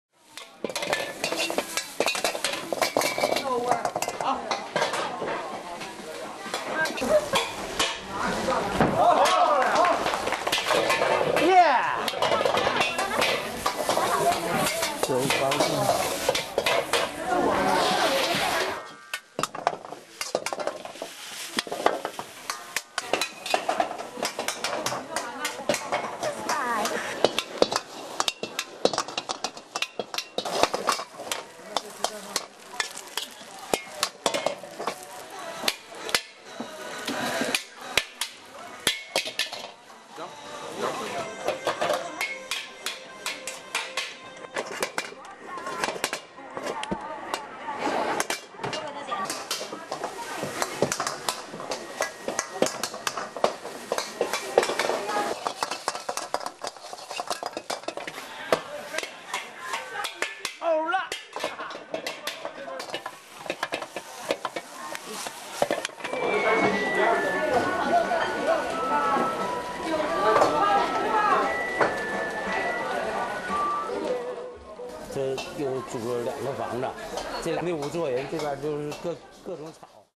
Chinese Chef cooks 15 meals at same time
A restaurant owner famous locally for his ability to cook 15 dishes simultaneously has been drawing holiday crowds in northeast China's Liaoning Province.

No comment | euronews: The most striking images from around the world without commentary.